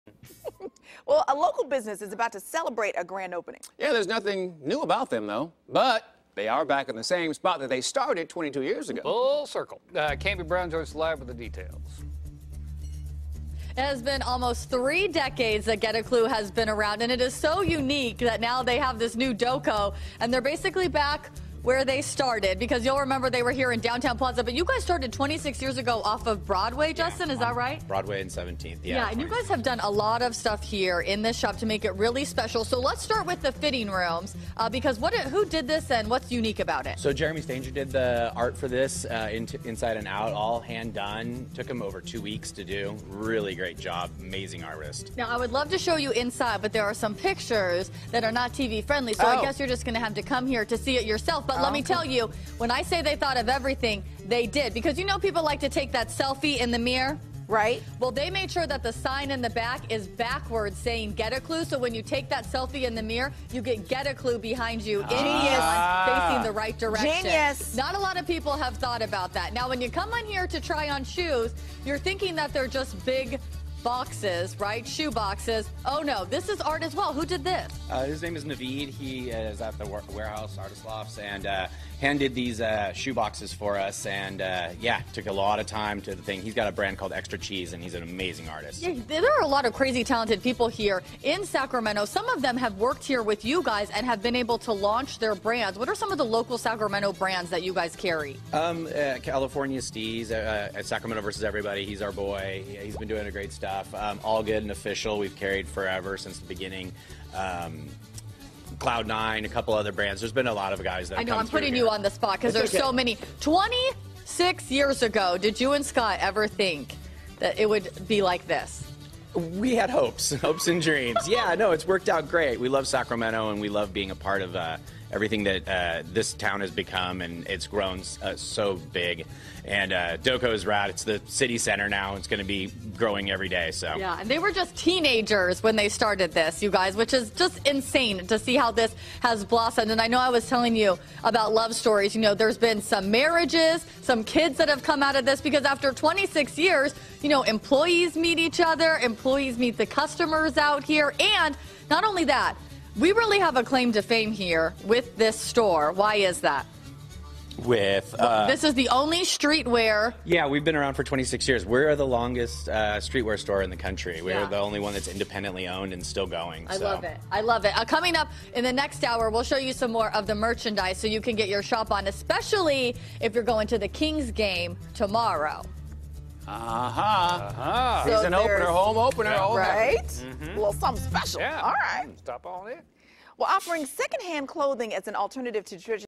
Getta Clue
Cambi Brown is at Getta Clue having their grand opening at the DOCO the same spot they are started in 22 years ago.